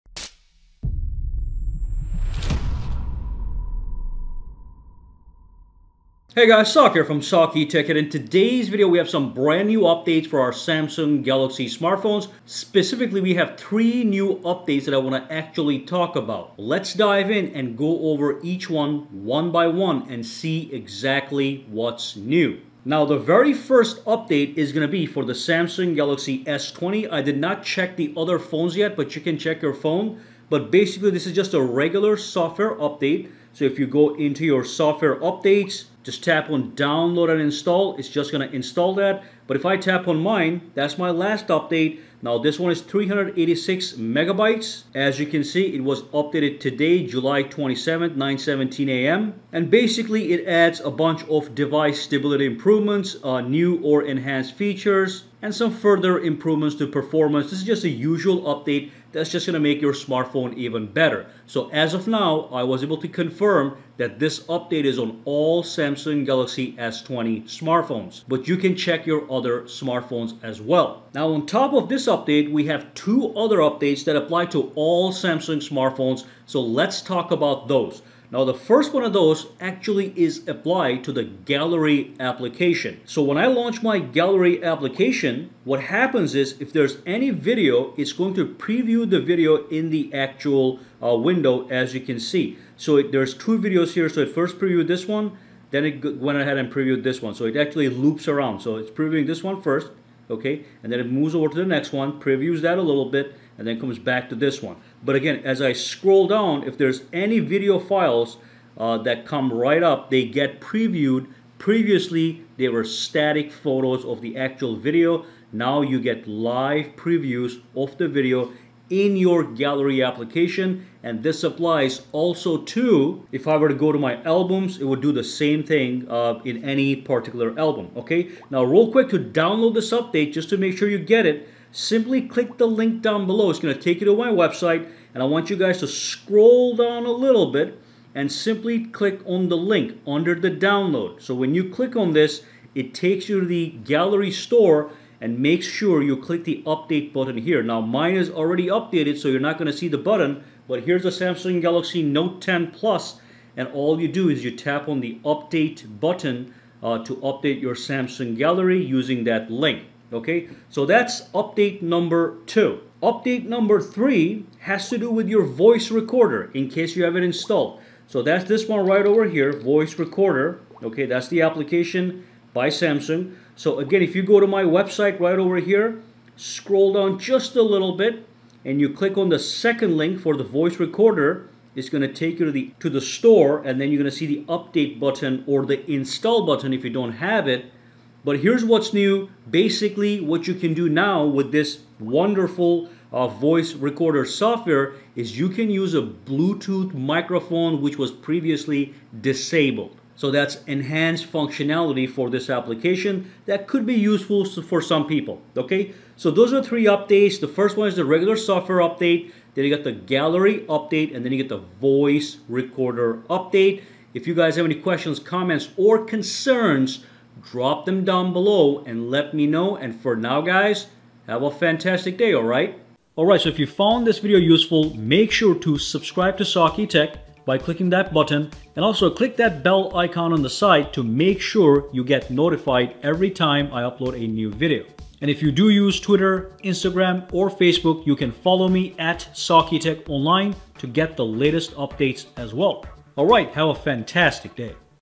NEW Software Update for Samsung Galaxy Smartphones - What's New? (New Features Added!)
Samsung Galaxy S20, S10, S10 Plus, Note 10 and 10 Plus get a brand new software updates. The Gallery app gets updated as well as the voice recorder app.

Let's dive in and see what kind of new improvements and features we have received on the Samsung One UI Smartphones.

Only certain regions and countries will get these particular new features.

Timestamps:

0:00 Intro to Samsung Galaxy Updates
0:22 First Update
1:10 Samsung Gallery App Update
2:08 How To Get These Software Updates
2:40 Samsung Voice Recorder Update
3:34 Outtro

Companies can send product to the following address:

Attn: SAKITECH
135 S Springfield Rd. Unit #681